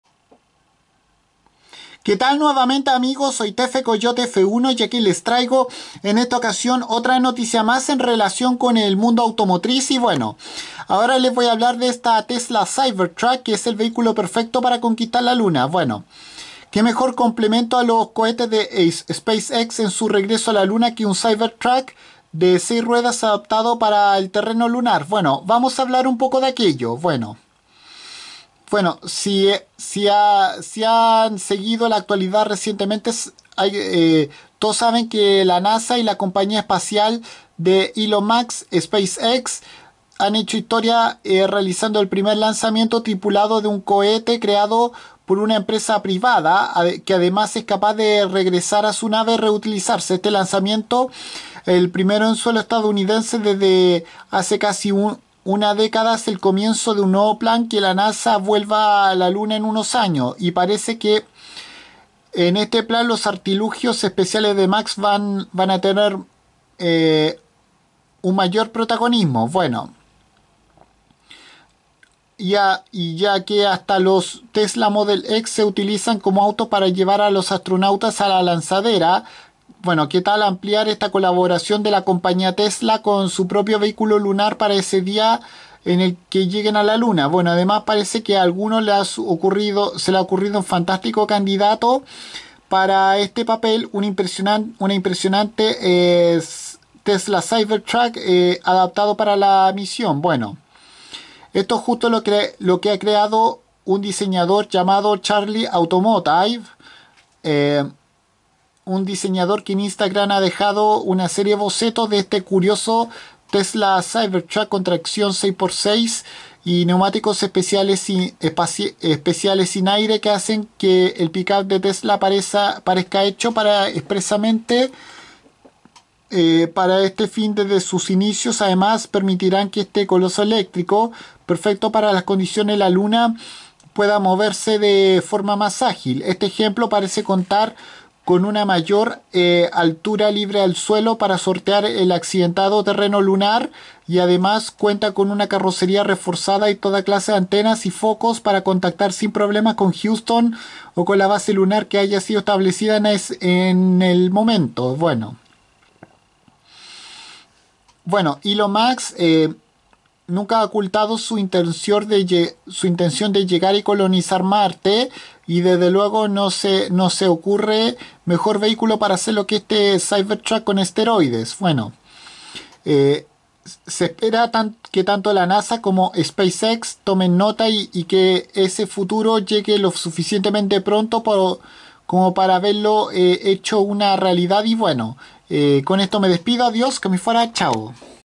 Esta Tesla Cybertruck ahora es el vehículo ideal para viajar a la Luna
Aquí les hablo de esta camioneta totalmente eléctrica de la compañía californiana Tesla que ahora posee una configuración de seis ruedas diseño que realizó a raíz de que el fundador y CEO de la marca Elon Musk estuviera vinculado a los cohetes tripulados de Space X(empresa también perteneciente al mencionado empresario)